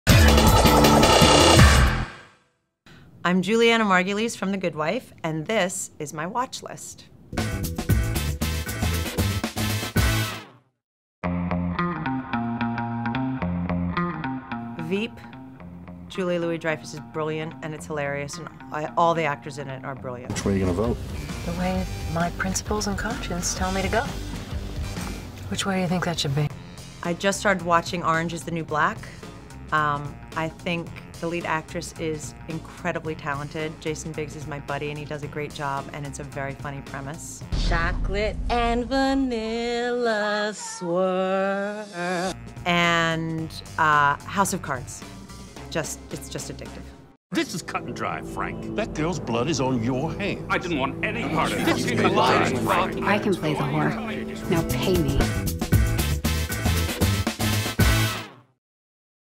Julianna Margulies shares her favorite TV shows on her Celebrity Watchlist
Which Netflix series does The Good Wife star love? Which HBO show does she call brilliant? Find out!